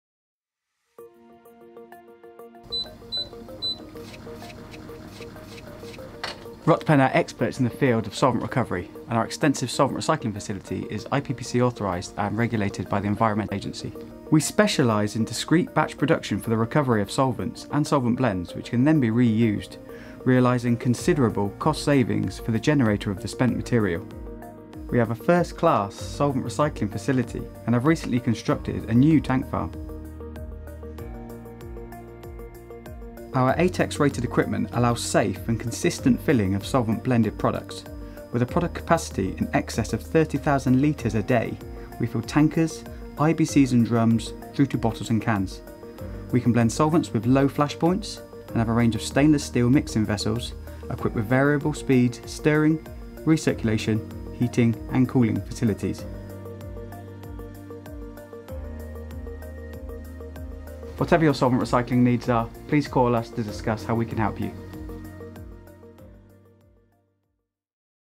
Solvent Recycling
At Rutpen we are experts in the recovery and recycling of solvent waste materials from production processes, such as automotive and printing, that allows the re-use of the solvent liquids.